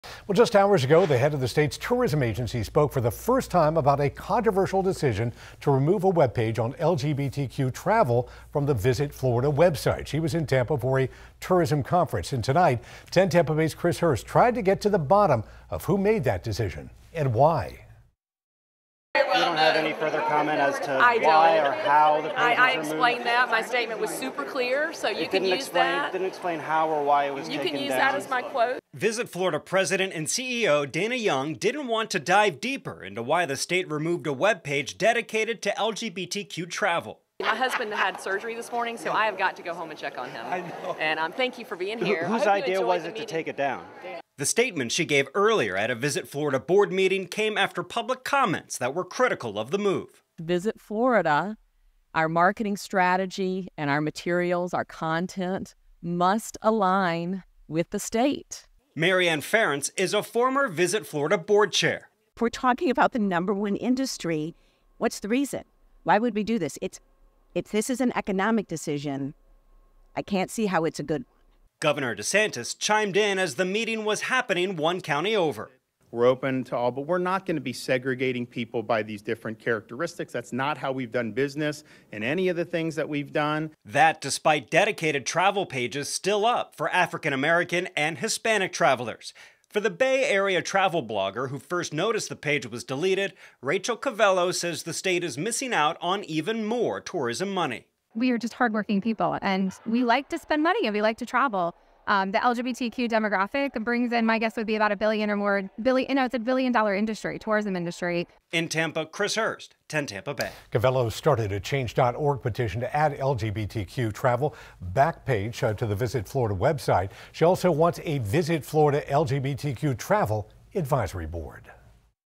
Visit Florida CEO addresses removal of LGBTQ pages on tourism website
The head of the state-funded tourism agency spoke for the first time about a controversial decision to remove a webpage on LGBTQ travel from the Visit Florida website. Visit Florida President and CEO Dana Young was in Tampa for a tourism conference.

“Visit Florida is a taxpayer-funded organization,” Young said to the Visit Florida Board of Directors. “And as such, Visit Florida, our marketing strategy and our materials, our content must align with the state. Florida has always been and will continue to be a very, very welcoming state."